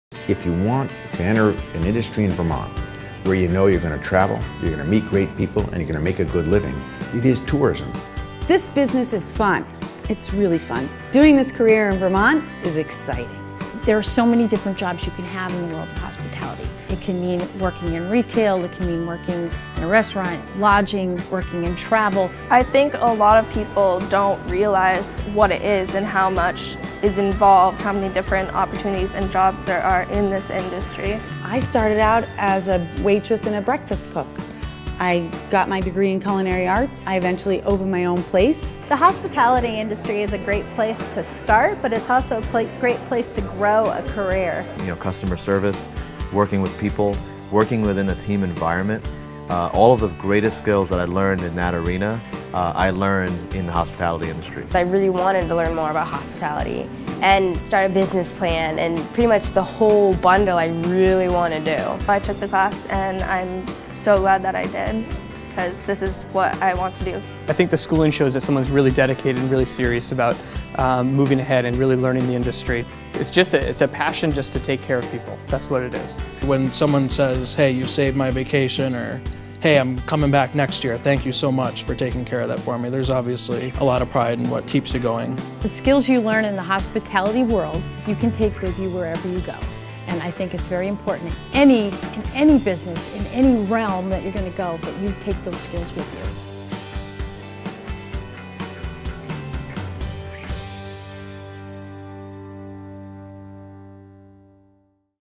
Tourism Education
Produced for the Vermont Department of Tourism and Marketing, this video is being used to highlight the opportunities that exist within the tourism industry for youth in high school